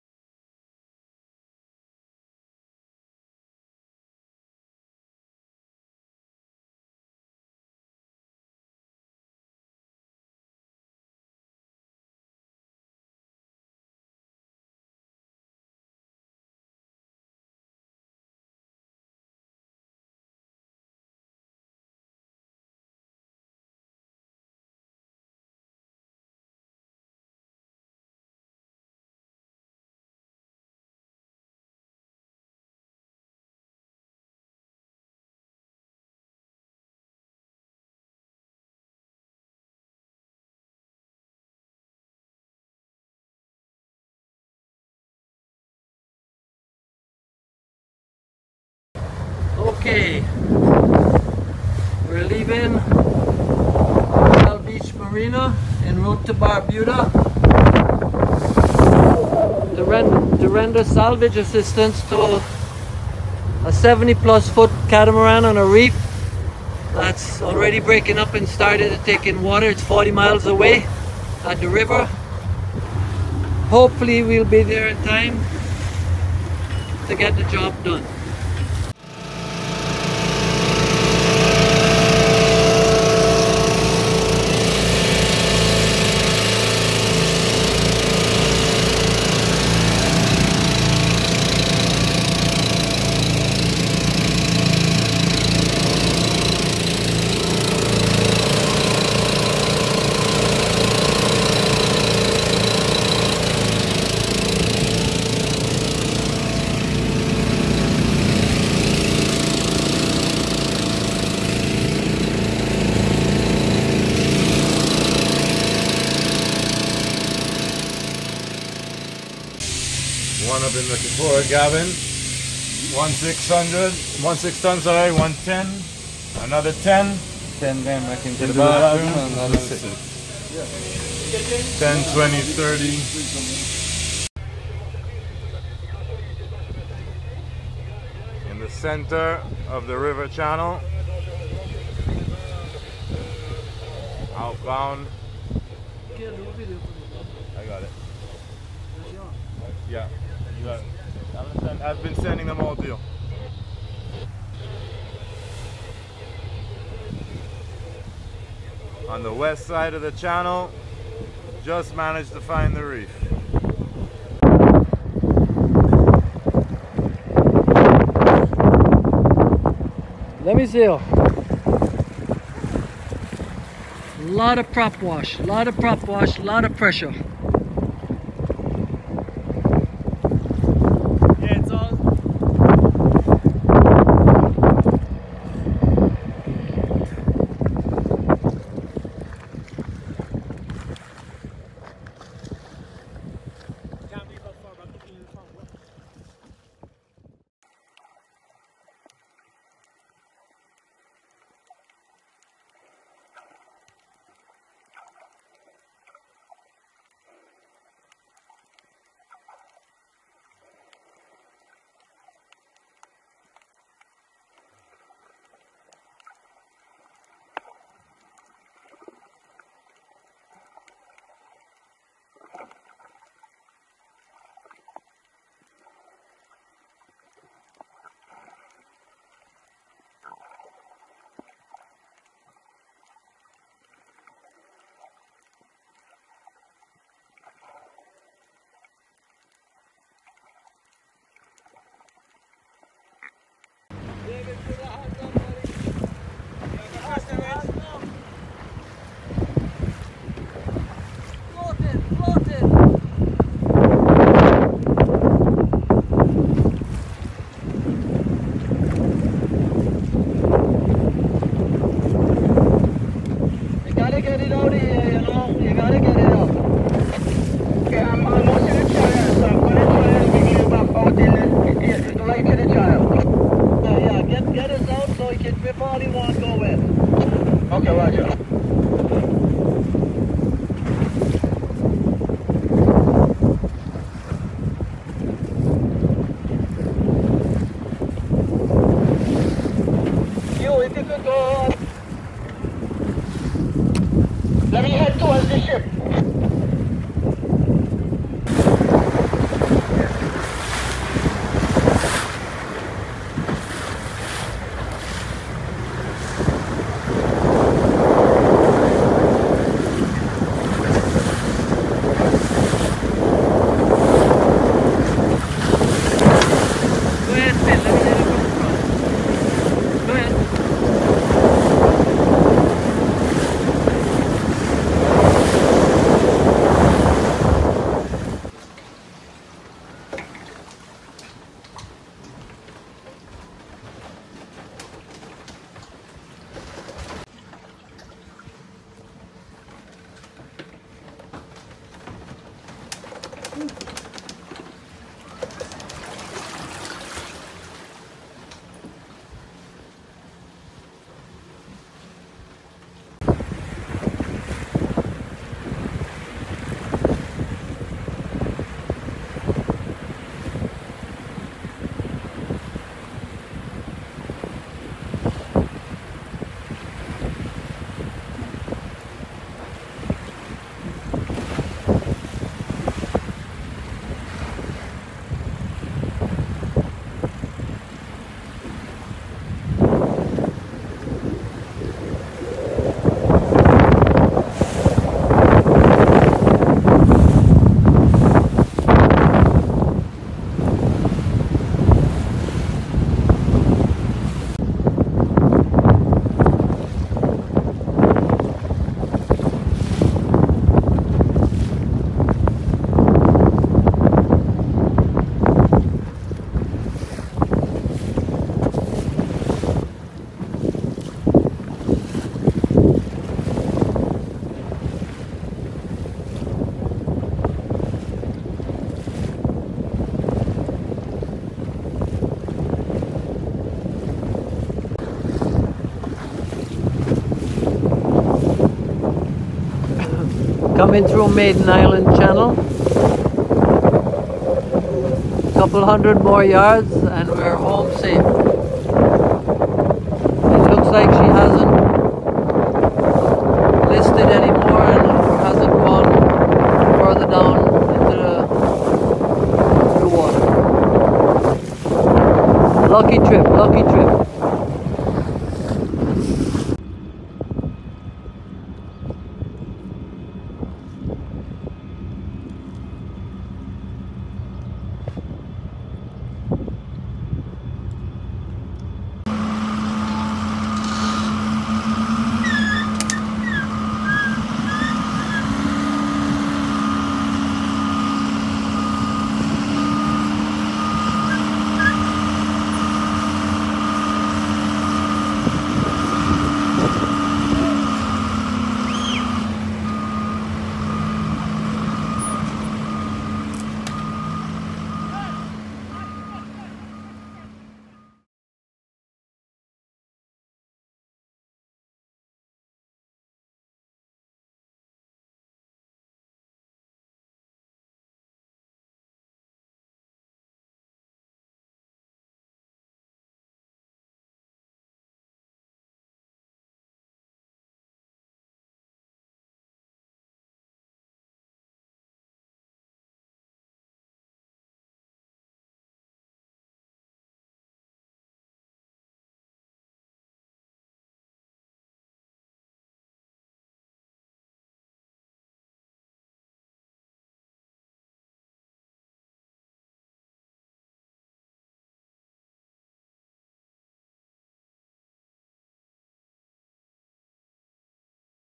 Mana Kai (Lagoon 77) Salvaged off Barbuda
The salvage of Mana Kai, a 77 foot Lagoon, off the coast of Barbuda.